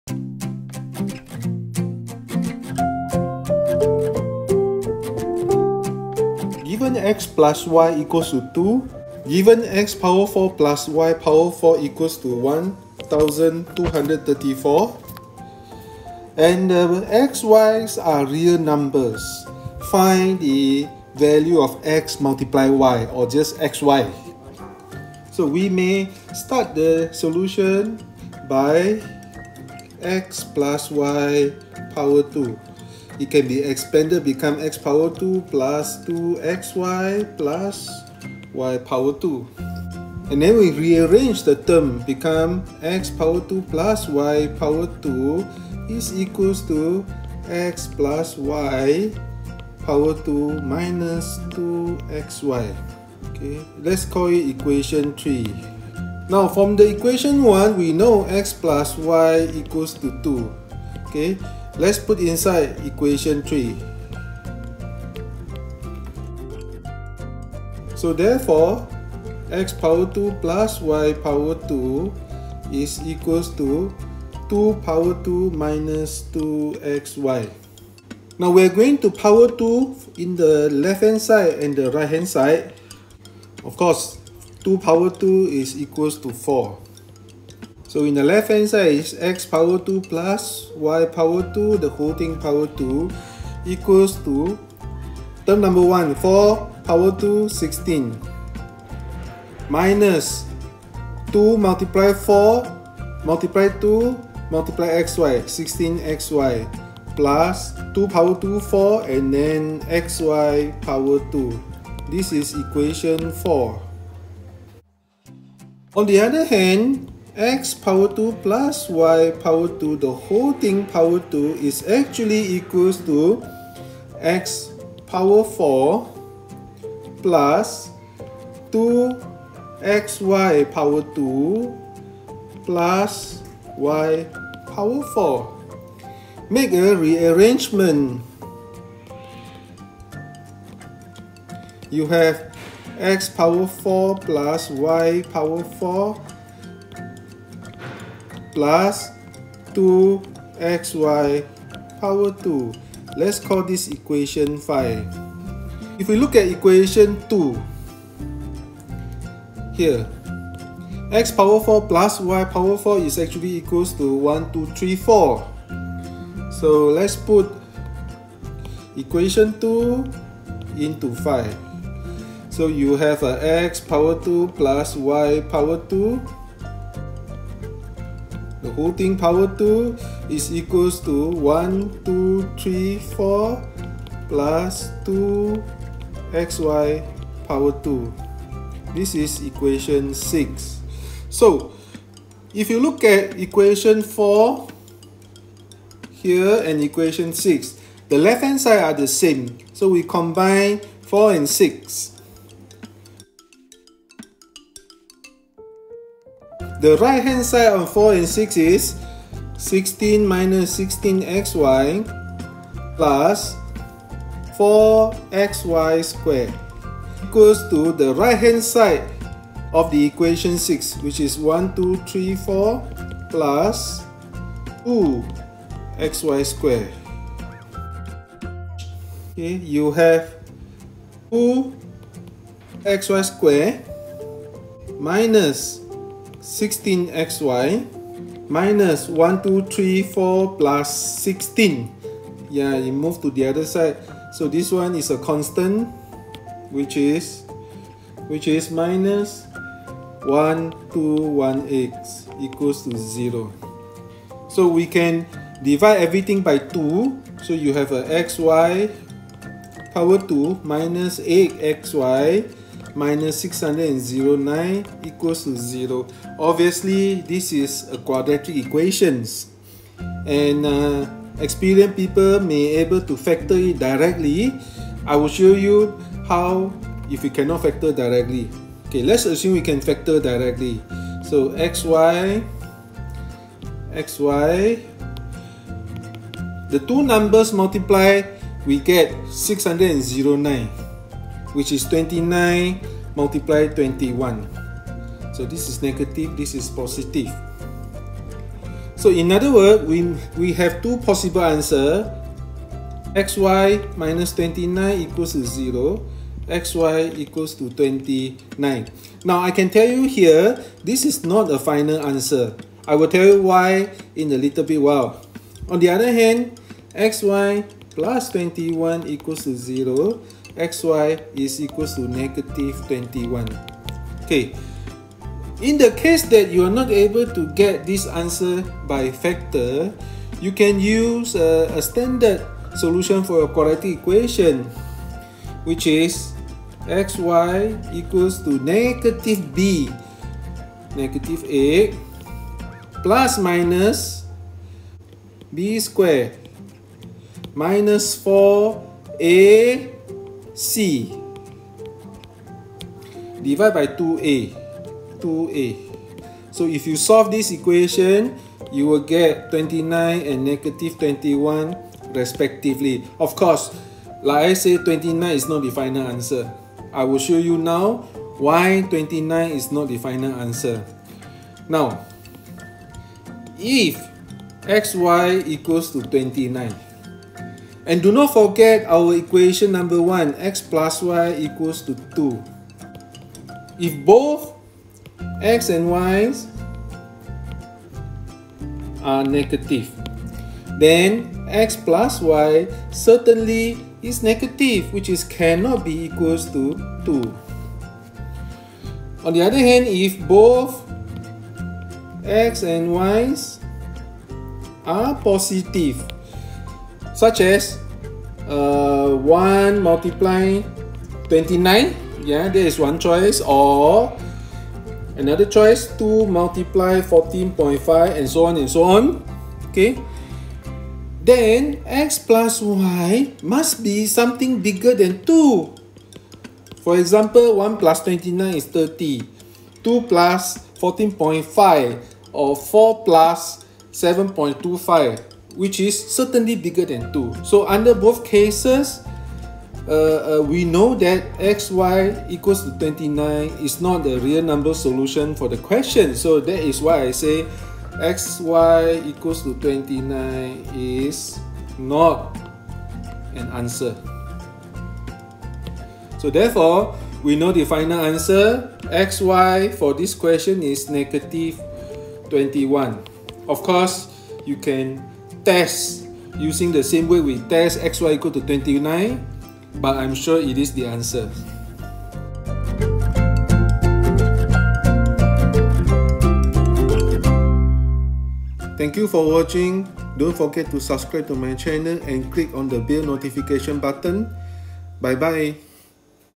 Given x + y = 2, x^4 + y^4 = 1234, find xy. A question from Stanford Math Tournament 2019.
Following are some of my videos on calculus, algebra, logarithm and trigonometry:
Calculus/Integration